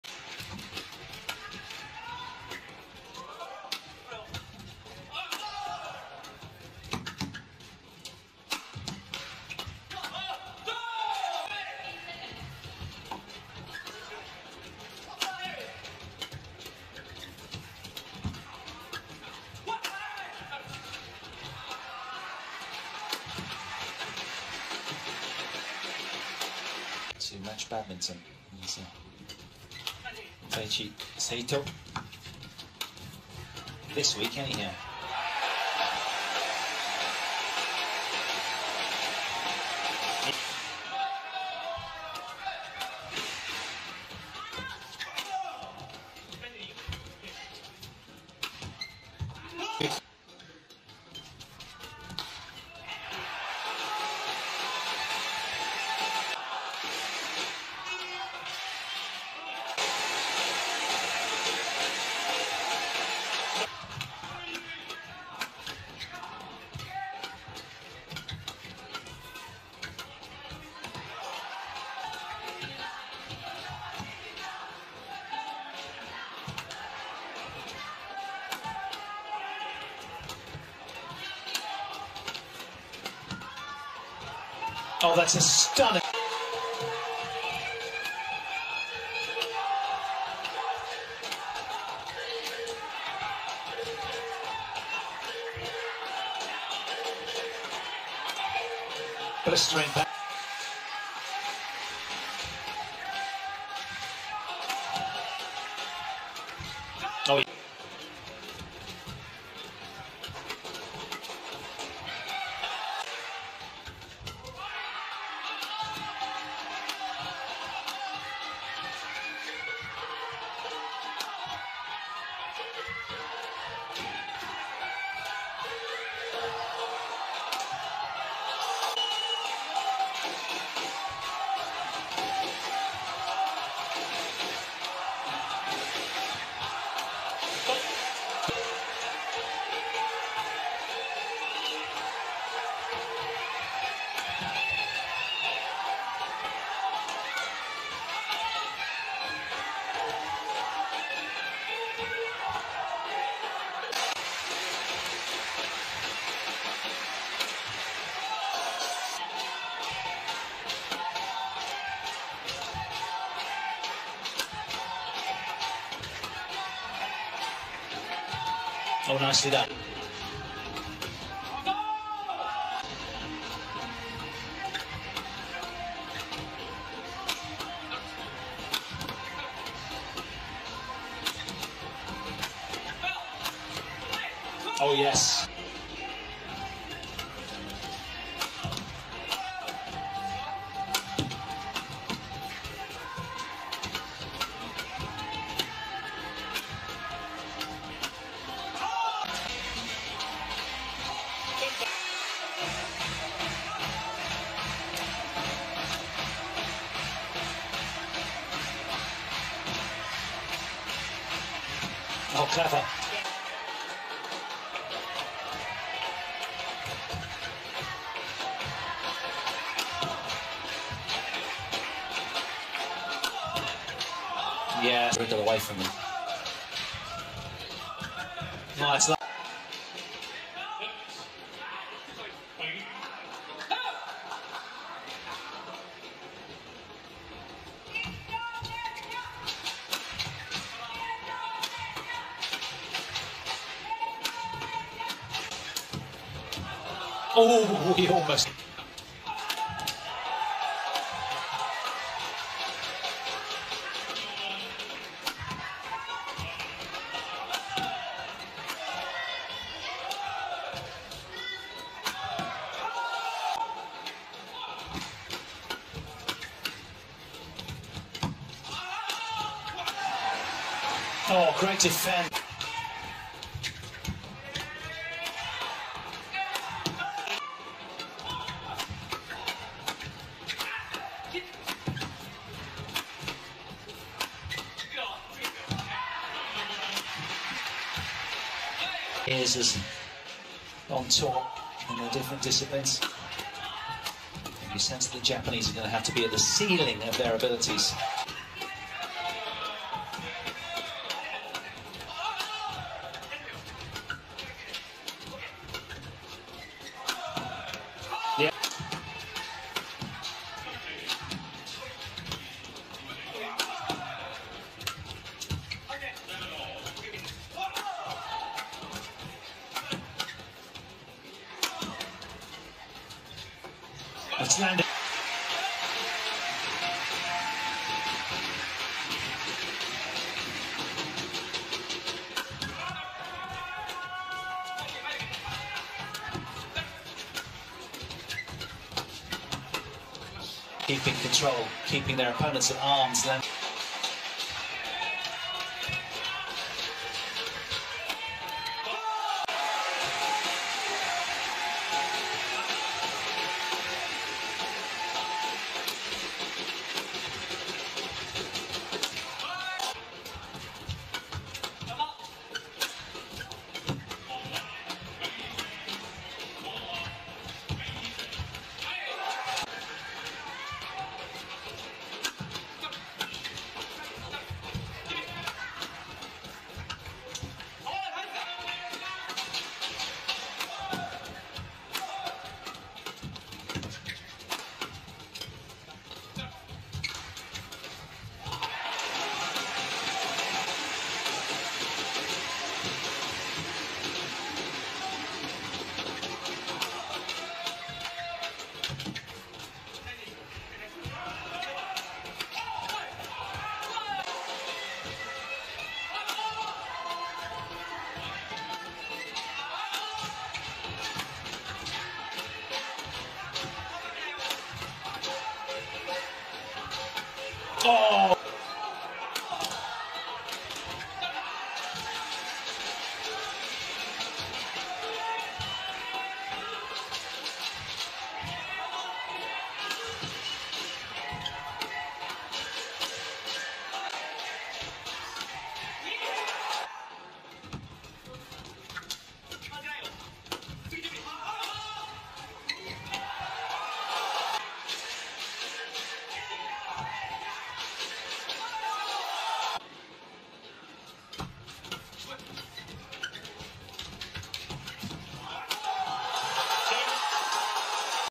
Cầu Lông SF Thomas Cup 2021 Koga/Saito vs Liu Cheng/Wang Yi Lyu (Japan vs China) trận đôi
Thomas Cup 2021 Koga/Saito vs Liu/Wang (Japan vs China)